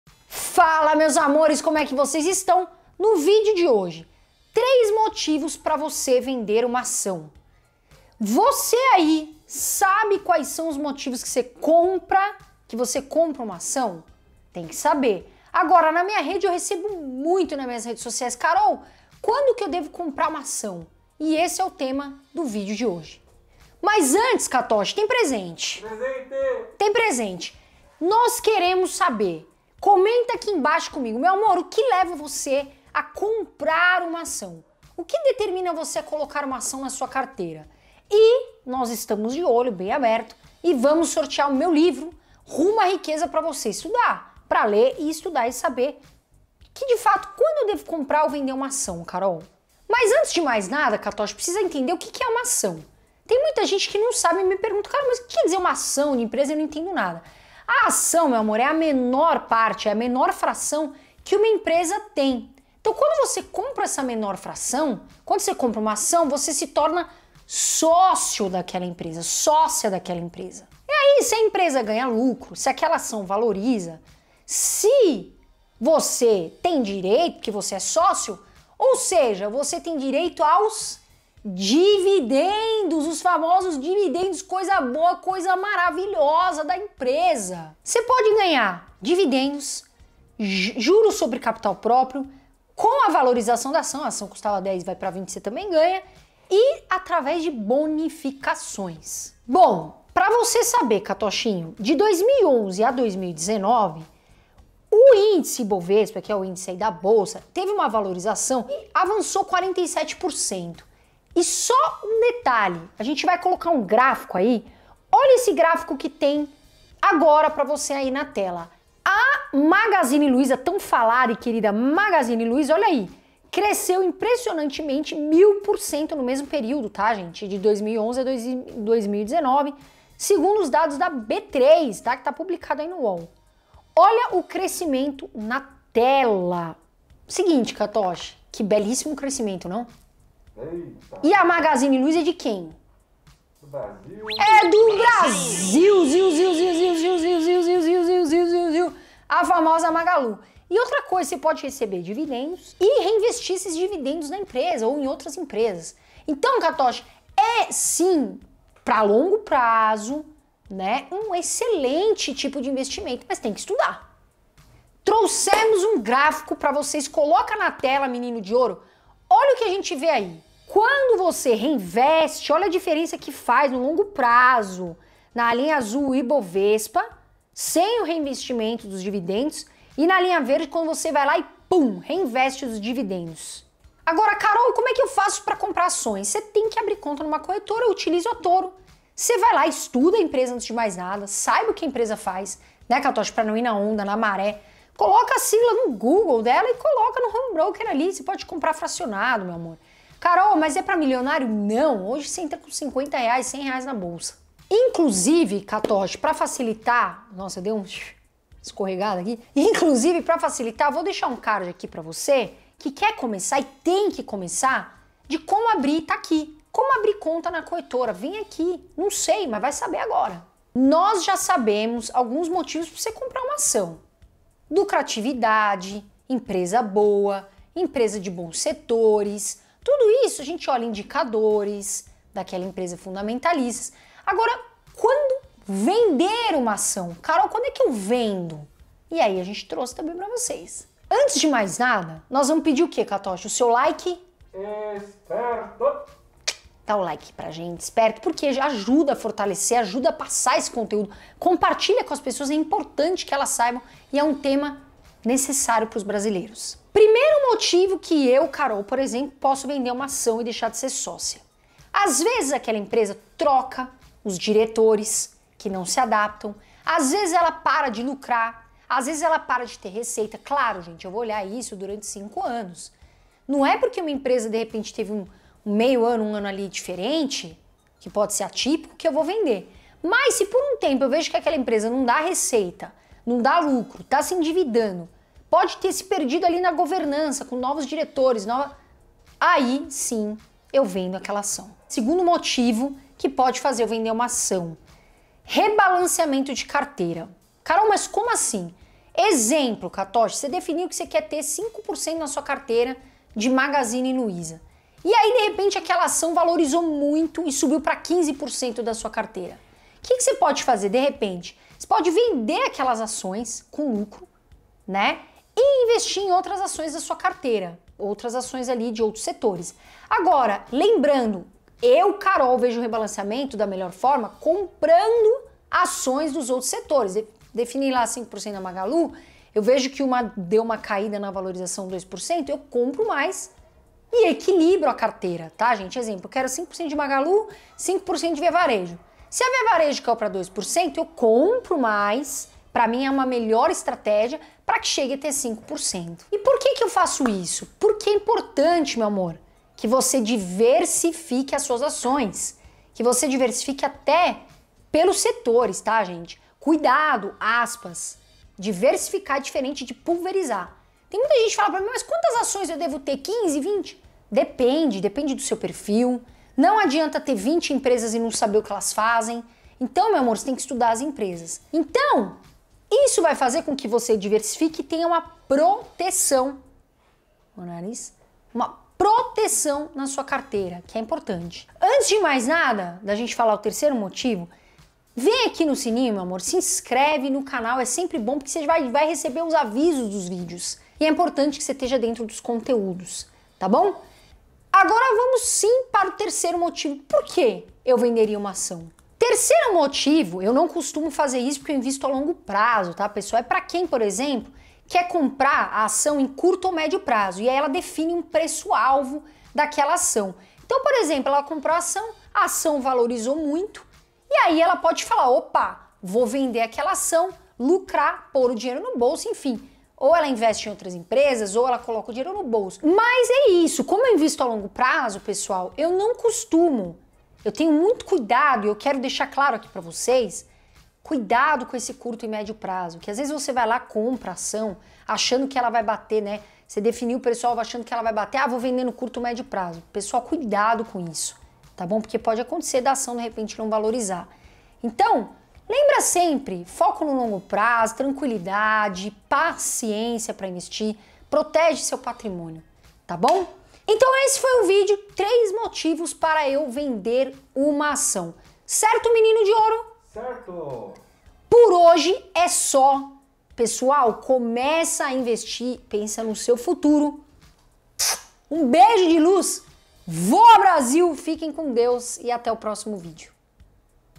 3 MOMENTOS CERTOS em que você precisa VENDER suas AÇÕES | QUANDO VENDER SUAS AÇÕES?
Quando preciso vender uma ação? Qual é a hora certa de vender ações? Nesse vídeo vou falar quando é o melhor momento para vender uma ação.

📌 Abra sua conta grátis na corretora Toro investimentos, taxa zero para investir e cashback direto na conta:

Nesse vídeo você vai ver:

00:00​ Tema do vídeo: Quando vender ações?
00:50 O que é a uma ação?
01:40 Ibovespa x Magalu
02:50 Retorno reinvestindo os dividendos
04:00 Quando vender uma ação?
04:15 1° Motivo: Perca dos fundamentos
04:50 2° Motivo: Rebalanceamento
06:30 3° Motivo: Preço-alvo